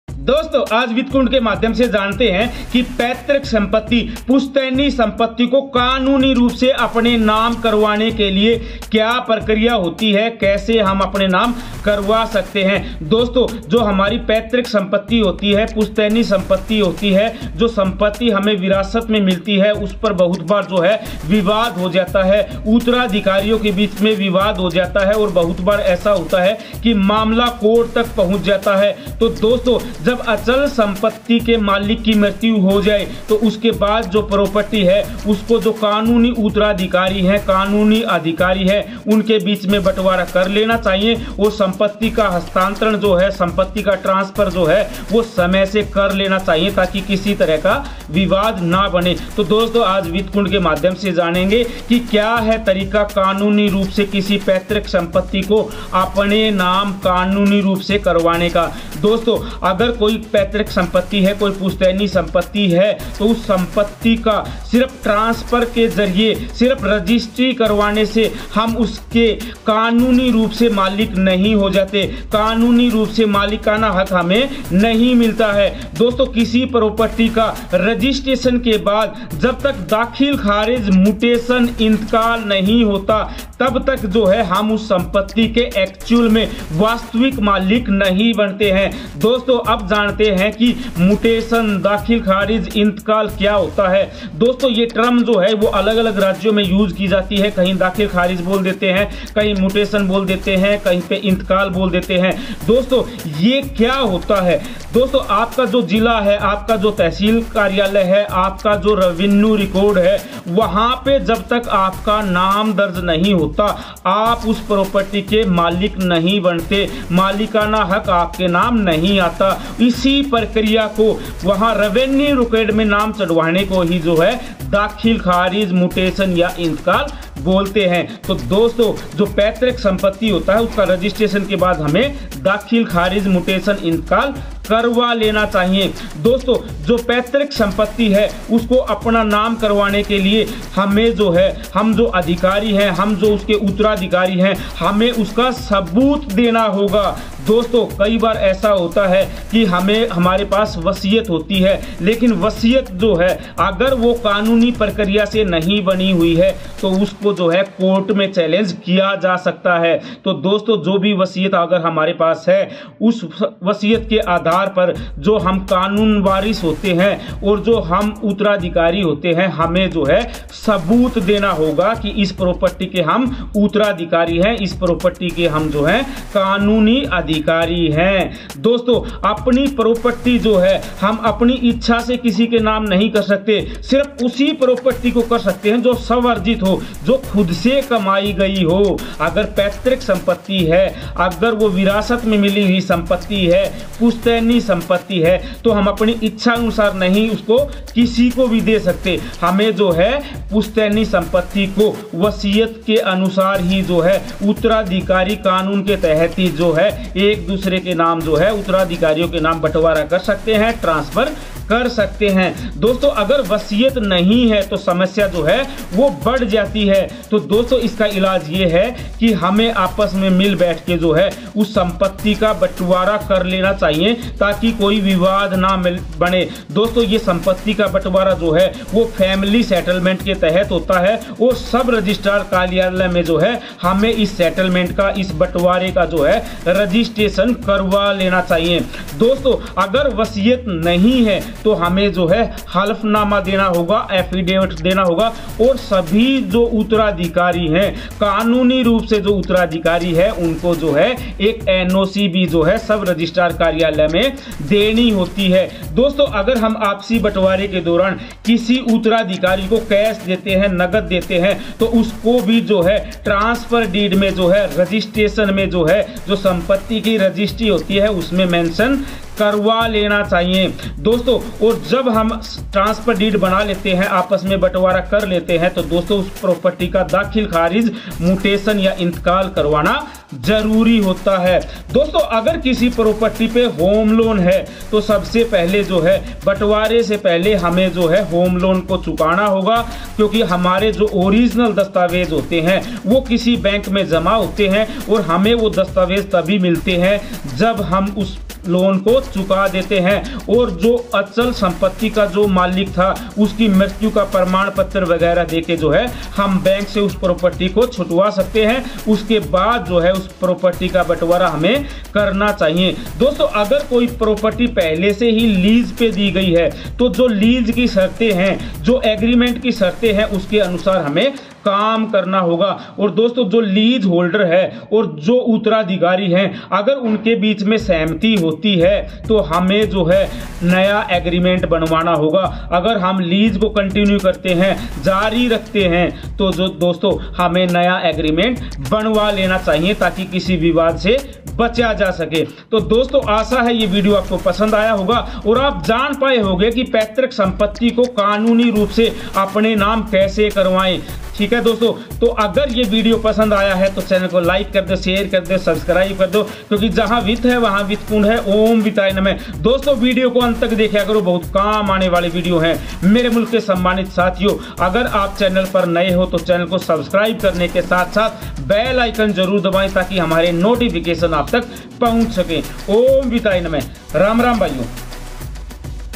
पैतृक संपत्ति को कानूनी तरीके से बिना विवाद अपने नाम करवाने के लिए करें ये काम, होगा जबरदस्त फायदा .
पैतृक संपत्ति को कानूनी तरीके और बिना विवाद अपने नाम करवाने के लिए करें ये काम, होगा फायदा :- वित्त कुंड

विरासत में मिली संपत्ति को हम कानूनी रूप से अपने नाम तब तक दर्ज नहीं कराते, जब तक किसी विवाद की आशंका न हो। विशेषज्ञों के मुताबिक, अचल संपत्ति के मालिक की मृत्यु होने के बाद की कानूनी उत्तराधिकारियों को इसे कानूनी रूप से अपने नाम कराना जरूरी है। यहां जानें इससे जुड़ी 7 खास बातें

1. संपत्ति हस्तांतरण की यह प्रक्रिया

संपत्ति हस्तांतरण की यह प्रक्रिया सिर्फ पंजीकरण मात्र से नहीं हो जाती। इसके लिए आपको दाखिल खारिज भी कराना पड़ता है। तभी आपका मालिकाना हक पूरा होता है। यह संपत्ति, कानूनी उत्तराधिकारियों की संख्या और अन्य वजहों पर निर्भर करता है कि इसके लिए क्या प्रक्रिया होगी।

2. संपत्ति अपने नाम ऐसे कराएं

पैतृक संपत्ति को अपने नाम कराने के लिए सबसे पहले आपको संपत्ति पर अधिकार और उत्तराधिकार का सबूत देना होगा। अगर संपत्ति के मालिक ने कोई वसीयत करा रखी है तो यह प्रक्रिया बेहद आसान हो जाती है। लेकिन वसीयत कानूनी प्रक्रिया के खिलाफ बनी होती है तो उसे कोर्ट में चुनौती दी जा सकती है।

मसलन, कोई शख्स मालिकाना हक वाली संपत्ति को तभी अपनी इच्छानुसार किसी को भी दे सकता है, जब उसने खुद इसे हासिल किया हो, न कि उसे भी यह विरासत में मिली हो। अगर उसे भी संपत्ति विरासत में मिली है तो उत्तराधिकार कानून लागू होता है।

3. वसीयत न होने पर समस्याएं ज्यादा

अगर कोई वसीयत नहीं है तो सबसे बेहतर होता है कि कानूनी उत्तराधिकारी आपस में सहमति से इसका बंटवारा कर लें। लॉ फर्म सिंह एंड एसोसिएट्स के संस्थापक साझेदार मनोज के. सिंह का कहना है कि परिवार के बीच हुए इस बंटवारे को फैमिली सेटलमेंट की तरह सब रजिस्ट्रार के कार्यालय में पंजीकृत कराना जरूरी है। इसके लिए संपत्ति के मालिकाना हक संबंधी दस्तावेज होना जरूरी है।

4. वसीयत न होने पर हलफनामा दें

वसीयत न होने पर एक हलफनामा तैयार कराना होगा, जिसमें सभी कानूनी वारिस या उत्तराधिकारियों का अनापत्ति प्रमाणपत्र (एनओसी) होना जरूरी है। अगर आपने अचल संपत्ति के सेटलमेंट के लिए किसी उत्तराधिकारी को कोई नकदी दी है तो उसका उल्लेख भी ट्रांसफर दस्तावेज में जरूर करें।

5. दाखिल-खारिज भी कराना चाहिए

संपत्ति के पंजीकरण के बाद उसका दाखिल-खारिज भी कराना चाहिए। यह राजस्व विभाग के आंकड़ों में किसी अचल संपत्ति का एक नाम से दूसरे नाम पर ट्रांसफर को दर्ज कराने के लिए आवश्यक है। प्रापर्टी टैक्स के भुगतान के लिए भी यह जरूरी है। साथ ही उस संपत्ति के साथ पानी, बिजली जैसे कनेक्शन भी दूसरे के नाम जुड़े होते हैं, उनके लिए भी दाखिल-खारिज आपके नाम होनी चाहिए। हर राज्य में दाखिल-खारिज का शुल्क भी अलग-अलग होता है।

6. प्रापर्टी पर होम लोन है तो चुकाना होगा

अगर जो प्रापर्टी आपके नाम होने जा रही है, उसे पर कोई होम लोन है तो आपको बाकी का पैसा चुकाना होगा। बकाये का भुगतान होते ही बैंक लोन क्लियरेंस सर्टिफिकेट के साथ आपको संपत्ति से जुड़े मूल दस्तावेज दे देता है। हालांकि अगर मृतक ने होम लोन इंश्योरेंस ले रखा है तो इसकी जरूरत नहीं पड़ती।

7. प्रापर्टी लीज पर है तो शर्तों का पालन जरूरी

अगर प्रापर्टी किसी को लीज पर दी गई है तो आपको उस एग्रीमेंट की शर्तों का पालन करना होगा। अगर कानूनी उत्तराधिकारी लीज को जारी रखना चाहते हैं तो लीज लेने वाले के साथ एक नया करार करना पड़ता है।